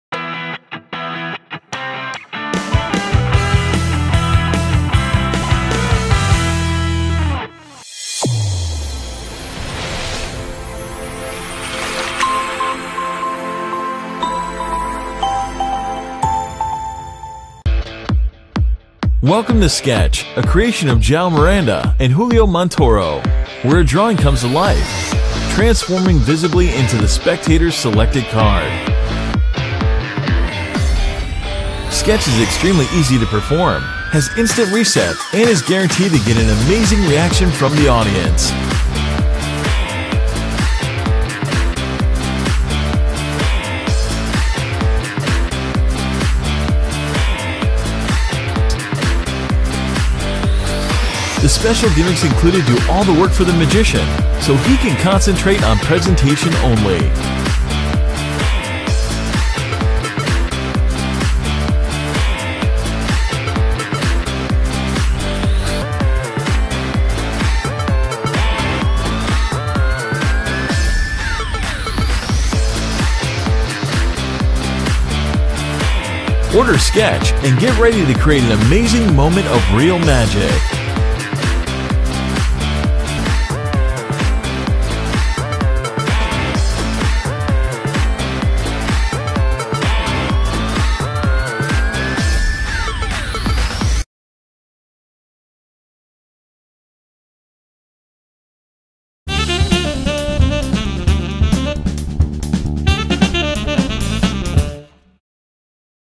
Sketch de Julio Montoro y Joao Miranda
Julio Montoro y Joao Miranda nos presenta Sketch! La carta elegida por el espectador se materializa visualmente de un dibujo hecho en un simple block!!